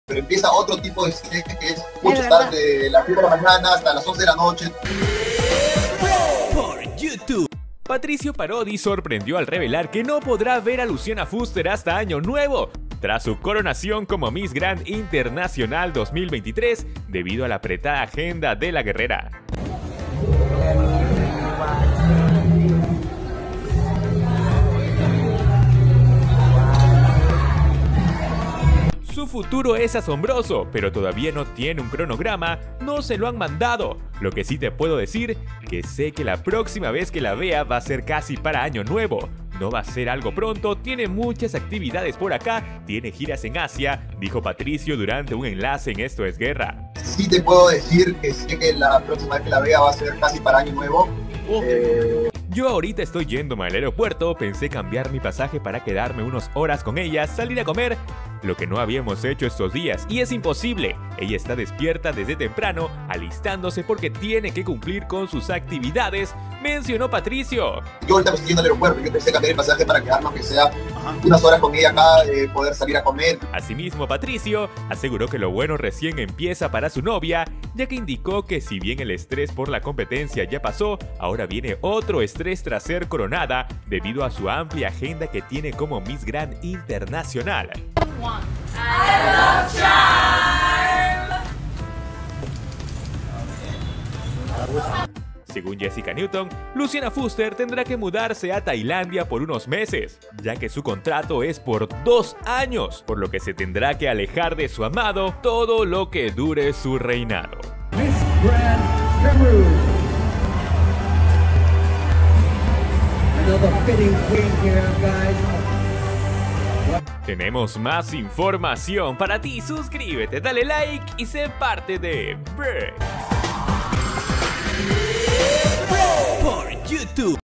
LUCIANA FUSTER: SU NOVIO PATRICIO PARODI SE ALEJA DE LA REINA TRAS GANAR LA CORONA
Patricio Parodi sorprendió al revelar que no podrá ver a Luciana Fuster hasta Año Nuevo tras su curación como Miss Grand International 2023, debido a la apretada agenda de la guerrera.